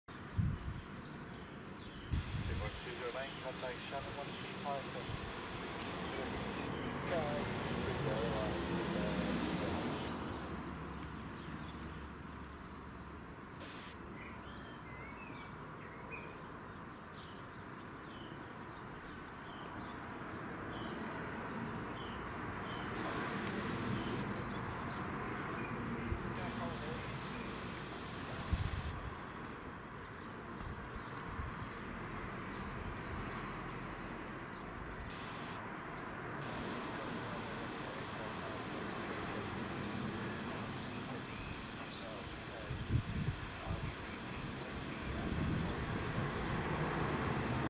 Flyover Boeing CC-177 Globemaster III (c17) Staffordshire today filmed in 4k..
Panasonic vx-980 4k 25i zoom